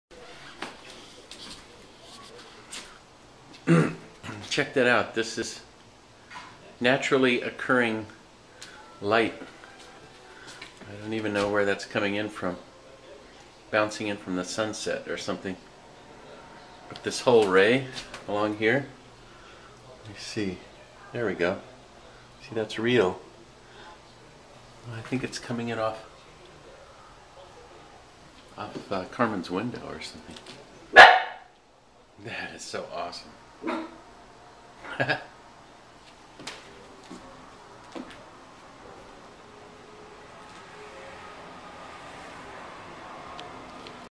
Eerie Light On My Brave Little Toaster Poster
Eerie light looked like it was painted right into the scene. Way cool!